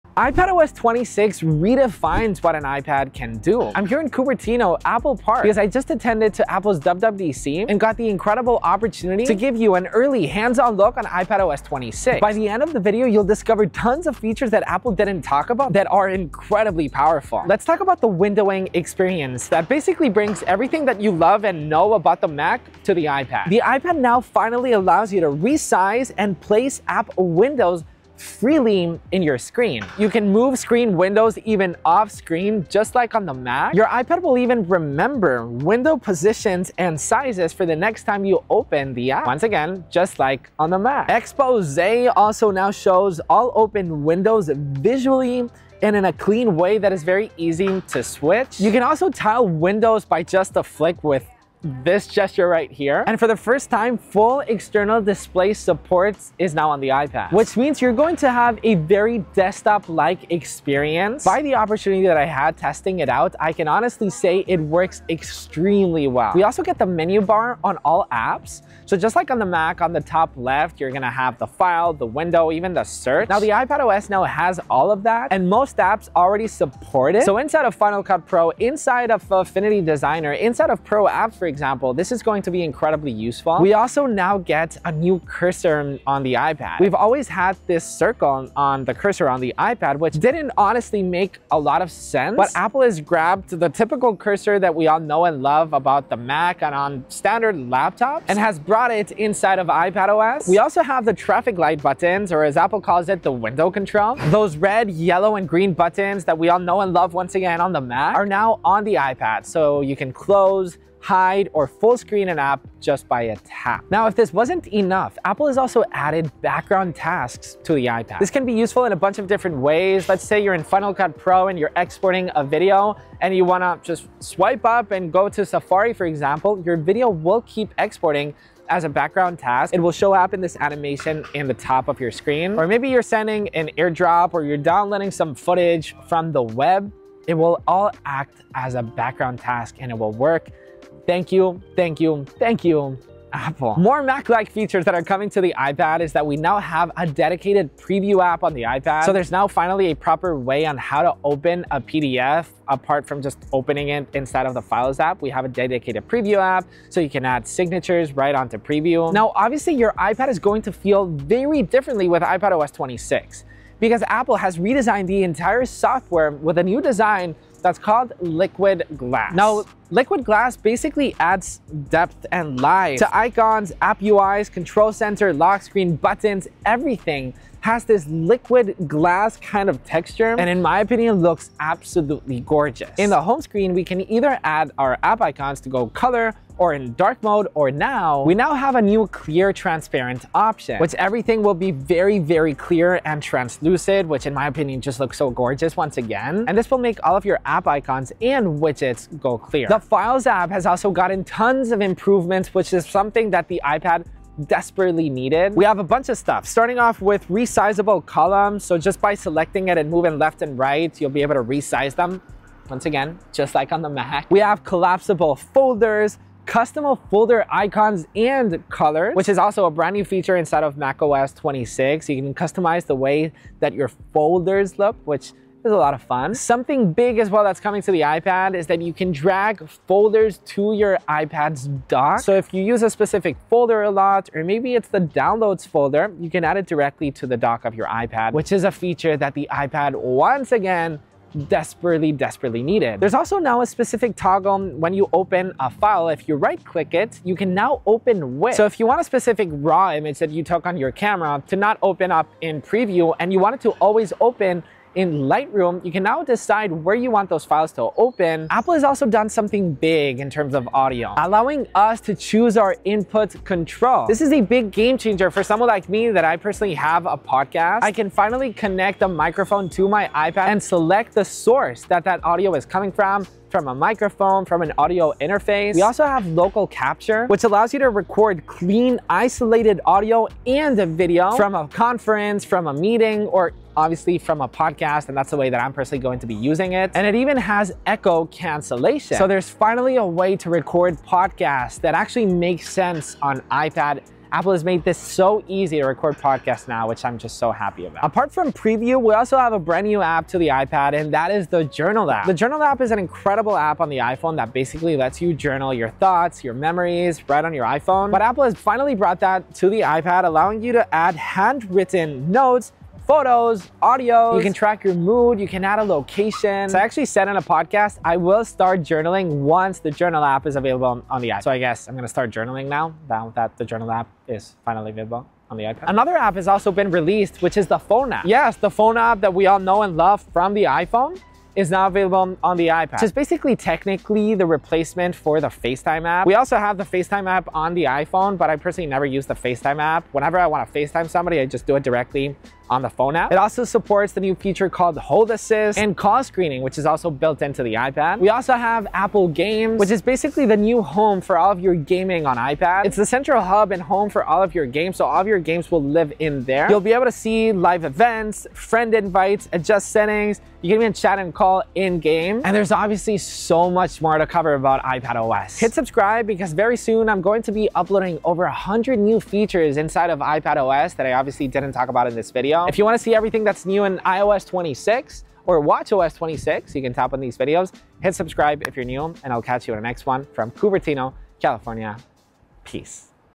iPadOS 26 Hands On – I Got It Early and It REPLACES the Mac
iPadOS 26 is finally here, and it’s packed with features that take the iPad experience to the next level—so much so, it’s starting to feel like a true Mac replacement. In this video, I dive into everything new, from Apple Intelligence features that bring on-device AI to your iPad, to a redesigned multitasking system that makes app switching faster and more intuitive than ever. You’ll also see new desktop-class Safari tools, enhanced external display support, a more powerful Files app, and improved Apple Pencil gestures that turn the iPad into a real productivity machine. Whether you're working, editing, or creating, iPadOS 26 brings the iPad closer than ever to replacing your Mac.